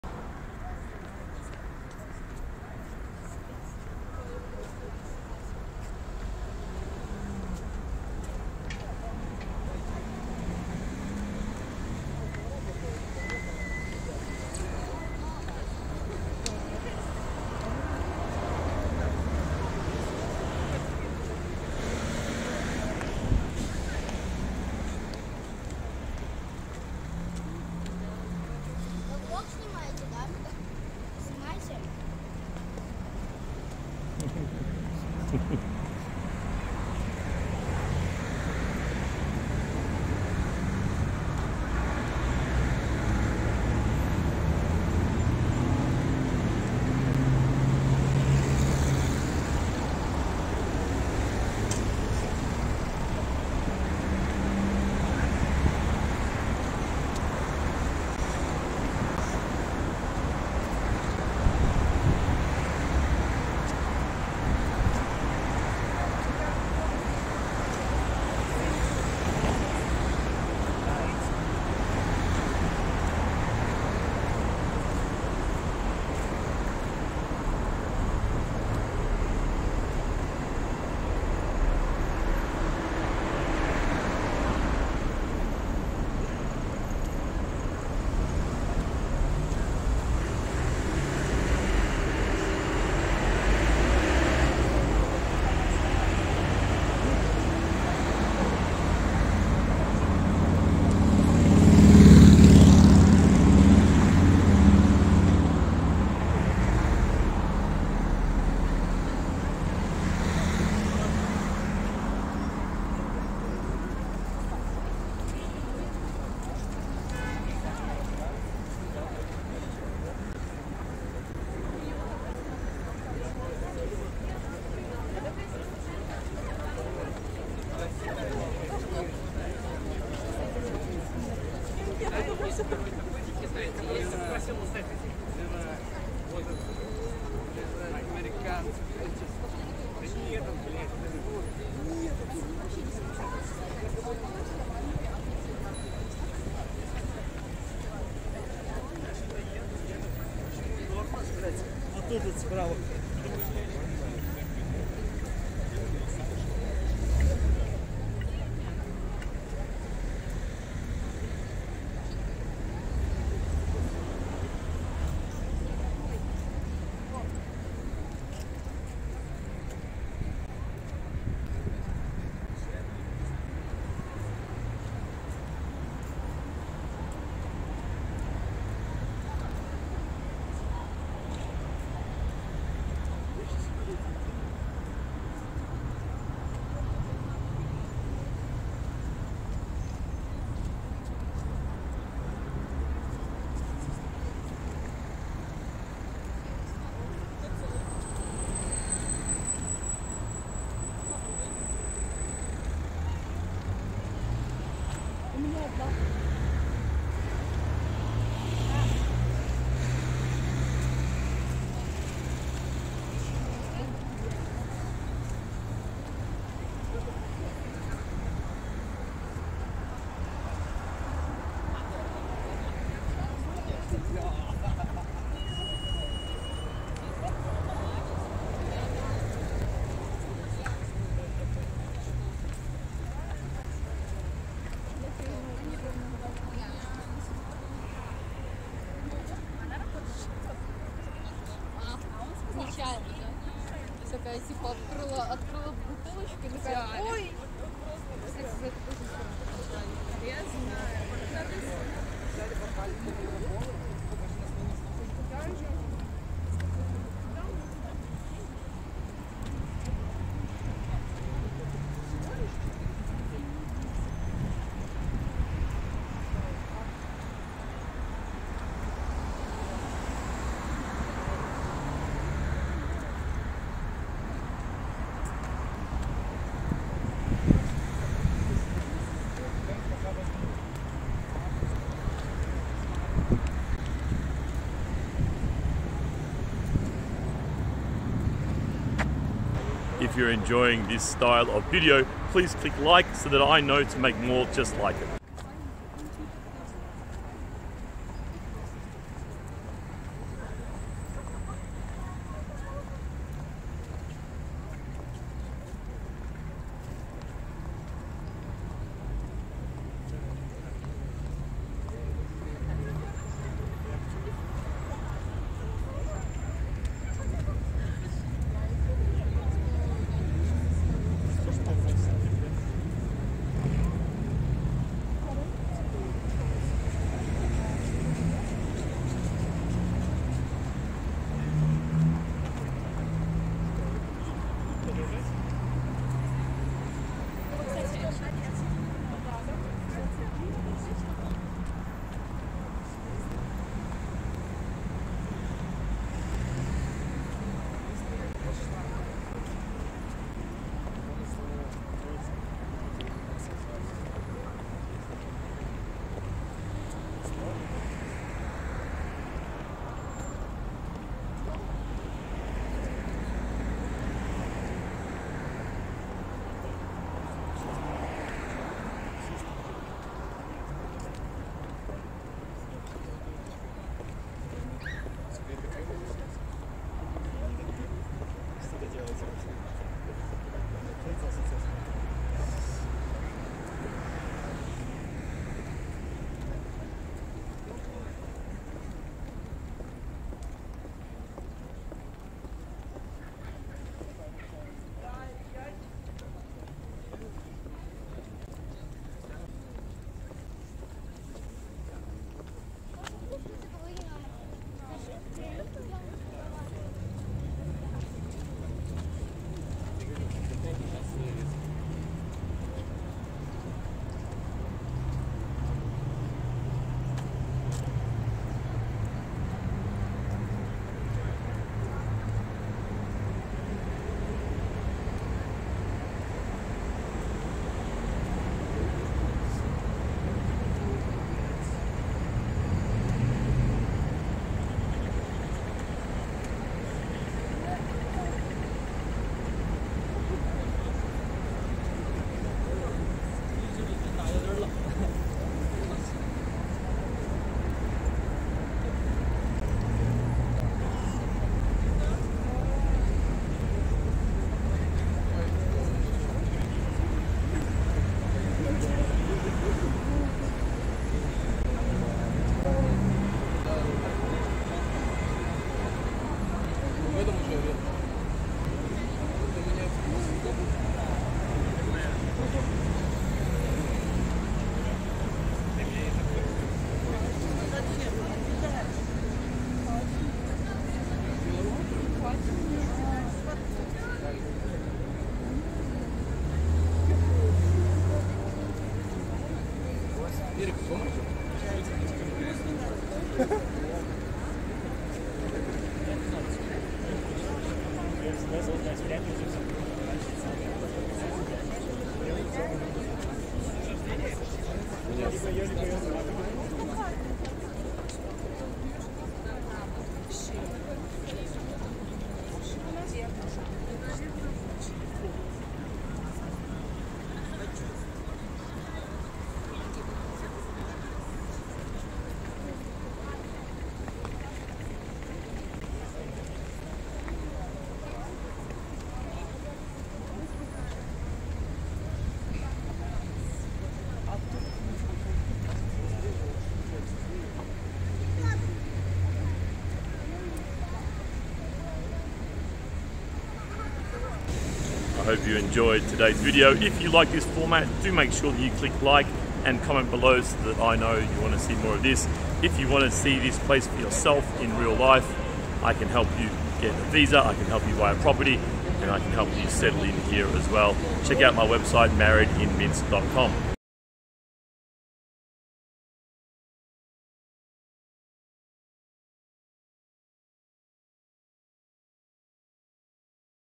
Street Walking in Minsk Belarus October 2022 Walking Tour
Come walking with me through Nemiga and see what the streets look like in Minsk which is the capital of Belarus. Nemiga is the central retail and business area inside Minsk which is the capital of Belarus. See the shops, the infrastructure and the people of Minsk, Belarus. I hope you enjoy this walking tour first hand!